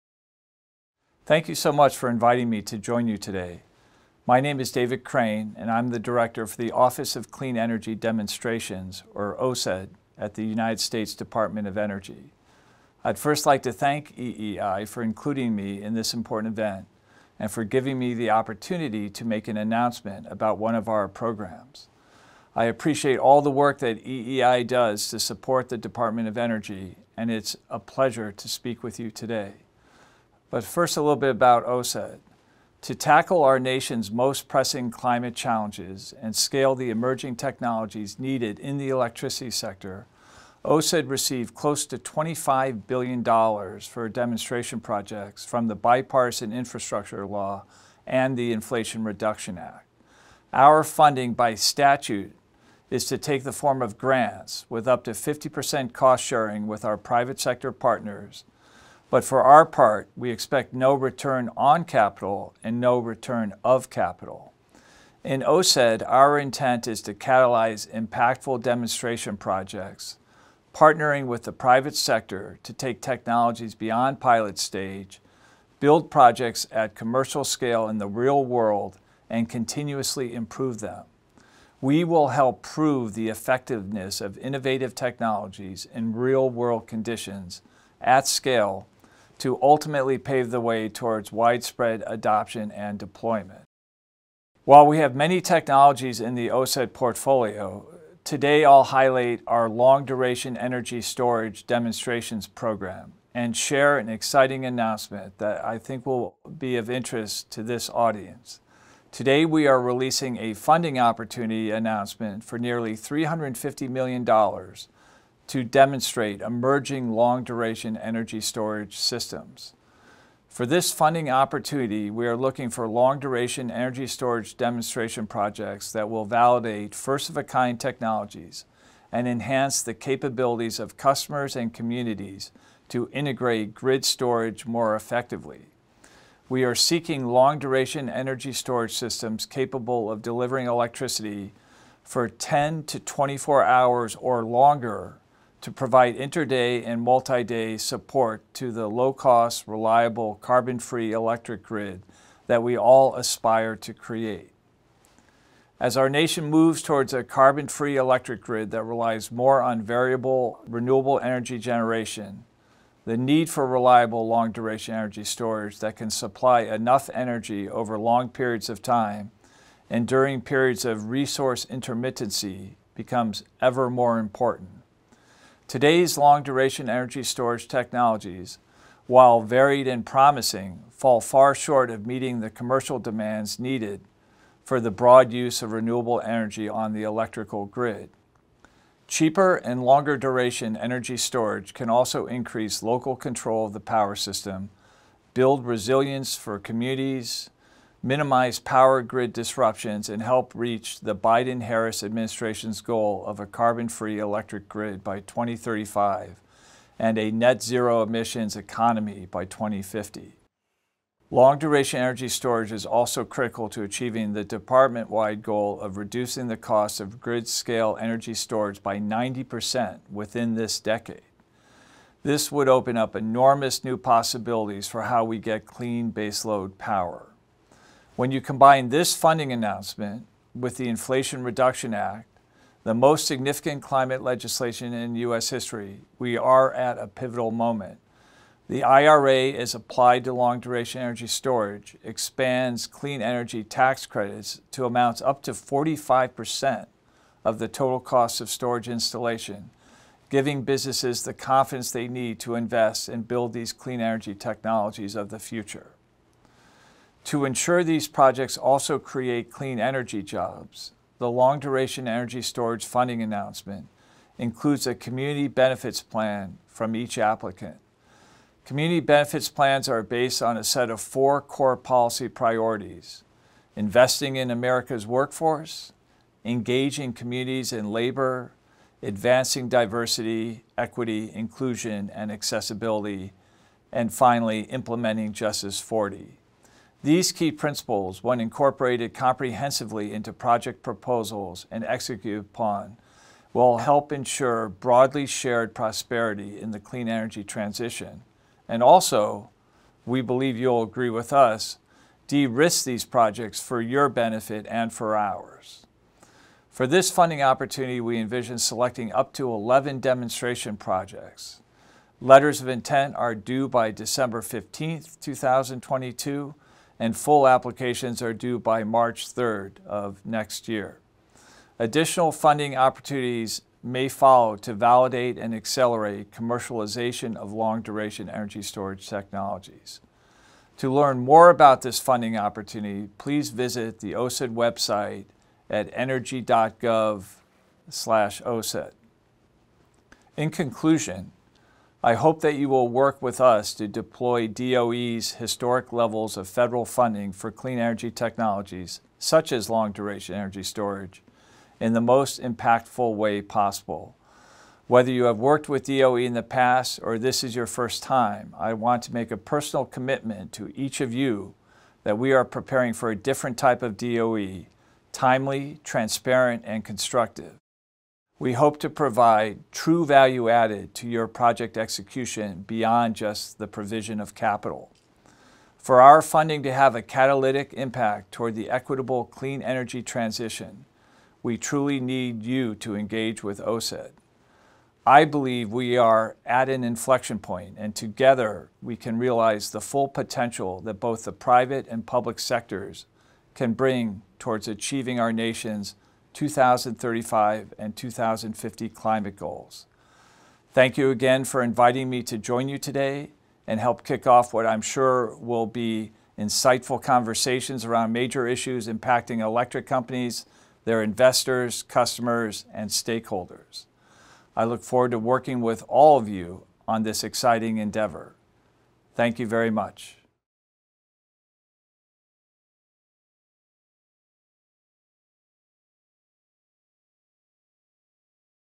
David Crane announces funding opportunity for Long-Duration Energy Storage Demonstrations
OCED Director David Crane announces nearly $350 million funding opportunity for Long-Duration Energy Storage Demonstrations at the 57th EEI Financial Conference